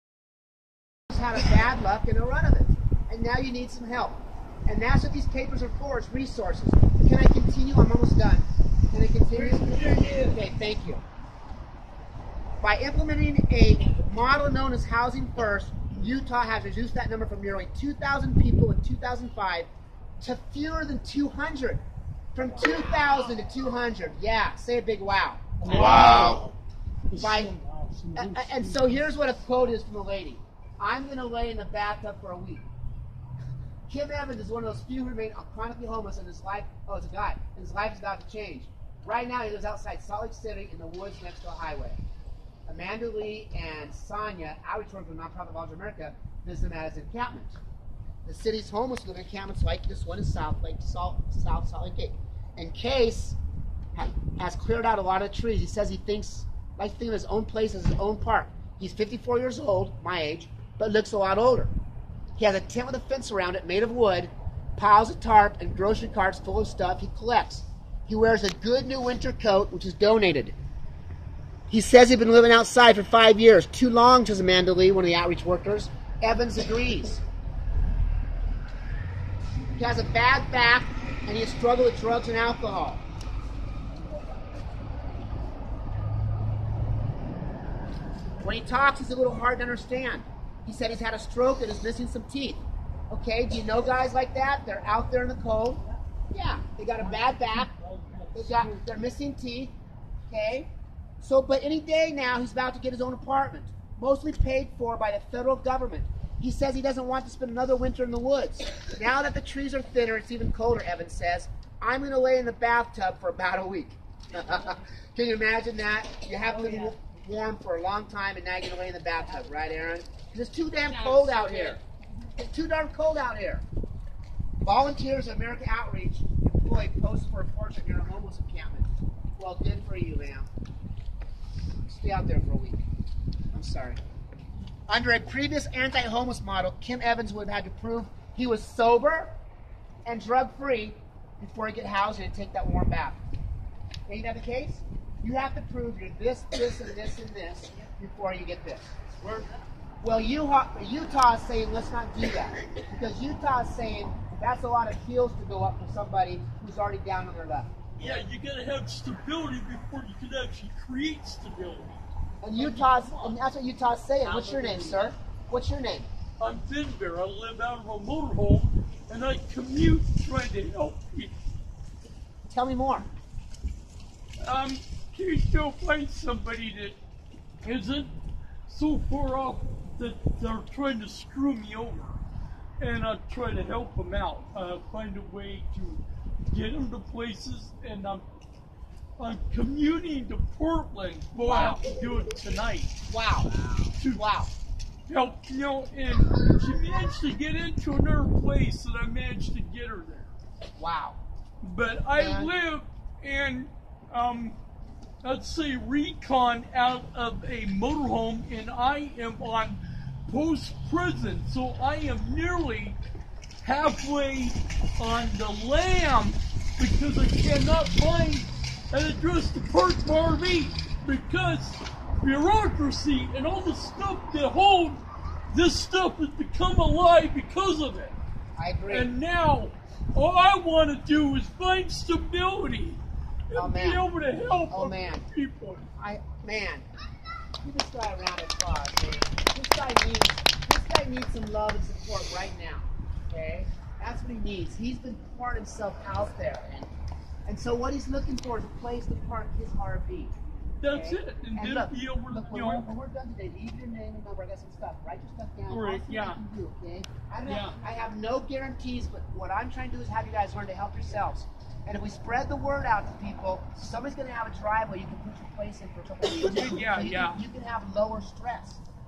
Keep us warm March(11)
Peaceful protest March and rally for homeless resource awareness in Salem Oregon, December, 2016